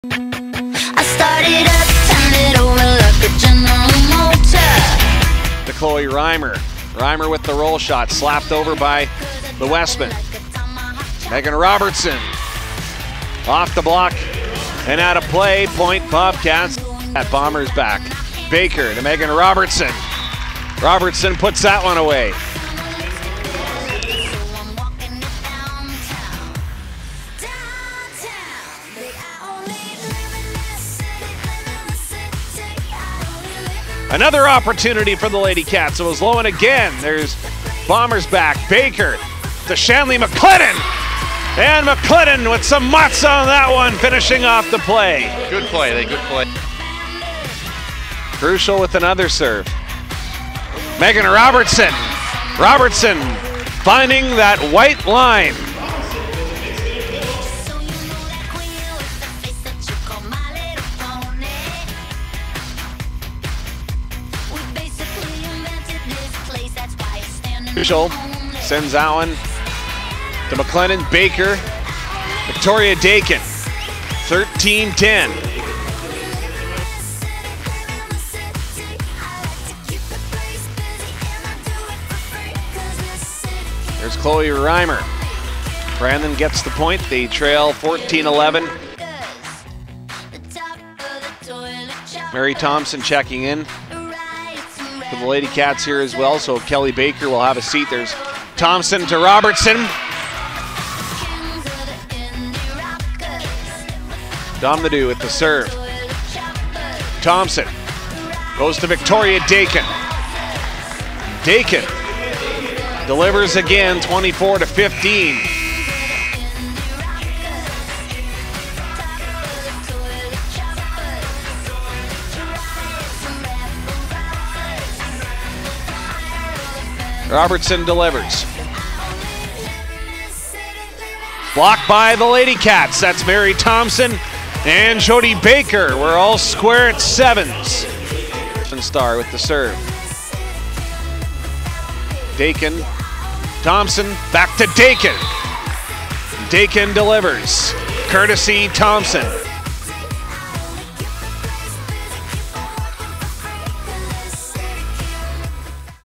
WVB vs Winnipeg - Nov 29
The last home game before the December break for the Bobcats women's volleyball team vs. the Winnipeg Wesmen.